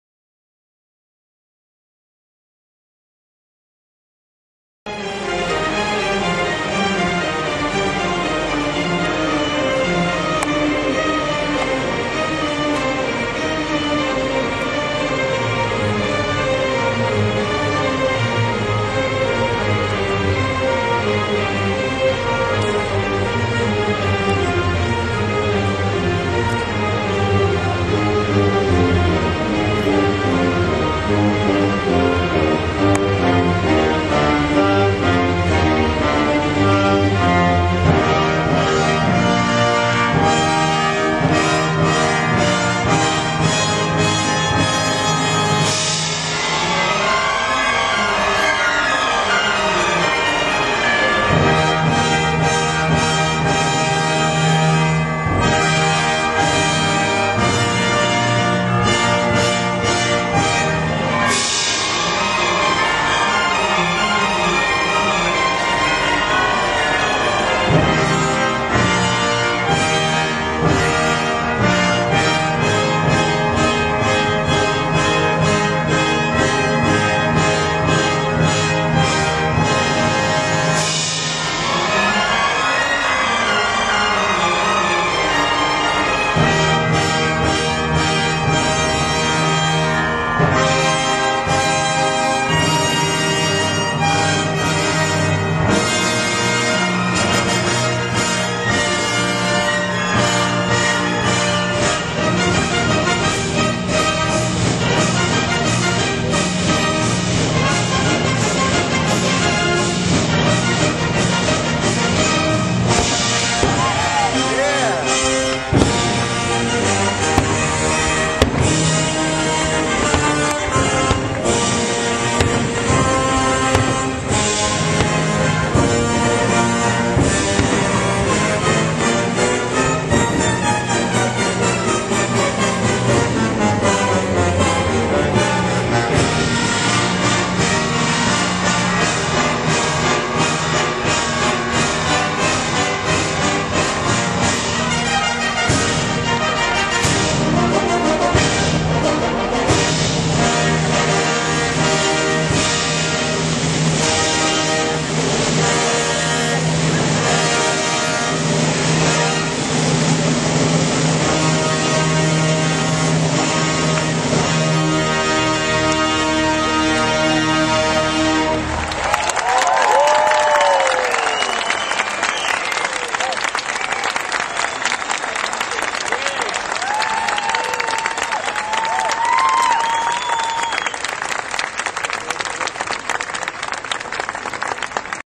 Grant Park Orchestra @ Petrillo Music Shell 070307
Conductor Christopher Bell and The Grant Park Orchestra performed the Indepdence Eve at the Petrillo Music Shell in Grant Park.